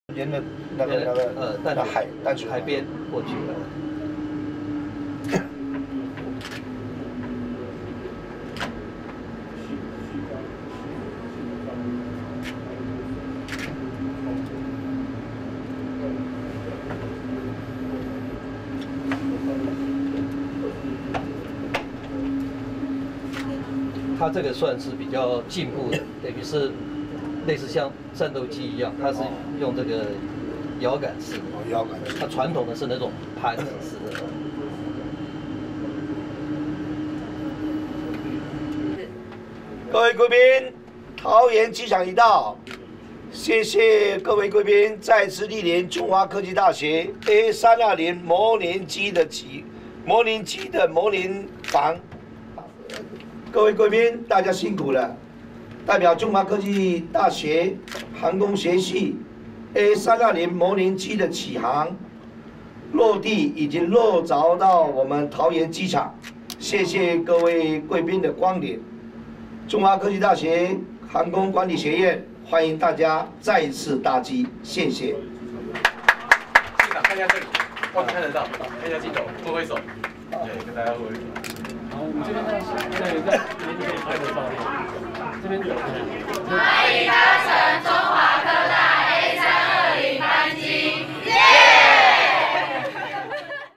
A320全罩式飛行模擬機-新竹縣縣長 邱鏡淳體驗
中華科技大學航空學院A320全罩式飛行模擬機-新竹縣縣長 邱鏡淳體驗
主辦單位  中華科技大學 航空學院
錄製單位  中華科技大學 遠距教學組
錄製指導老師  李柏堅
錄製行政協助  黃美玉
影片錄製  楊智森
影片後製  楊智森